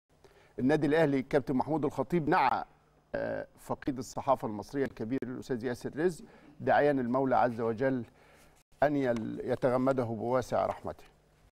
الكابتن الخطيب ينعى الكاتب الصحفي الكبير ياسر رزق
الكابتن الخطيب ينعى الكاتب الصحفي الكبير ياسر رزق | ملك وكتابة